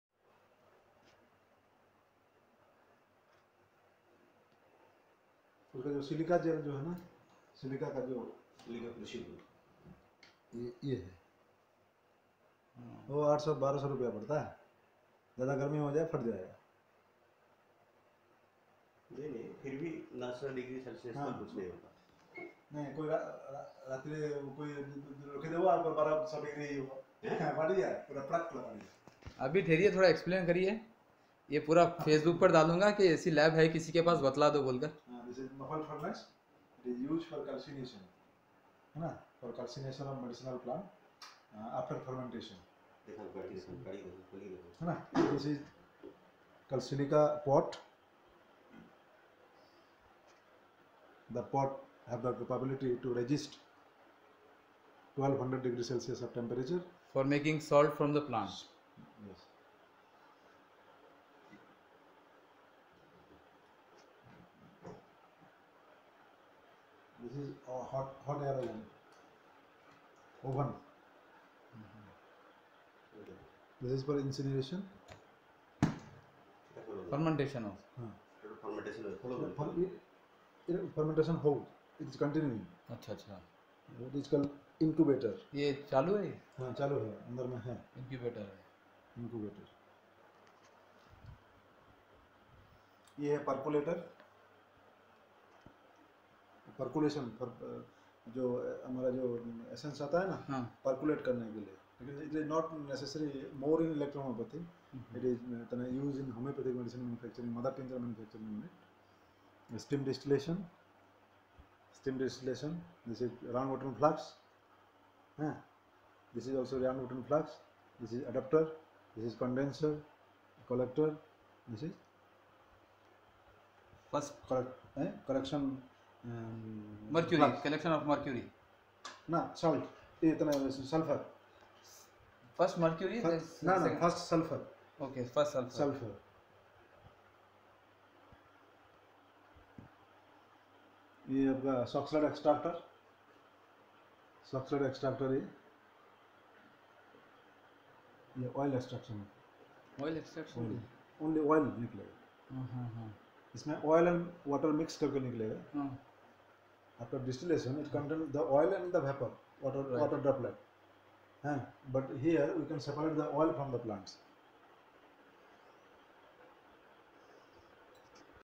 Dr susanta padhee.... Mfg of spagyric essence
How to prepare spagyric essence ..and their required instrument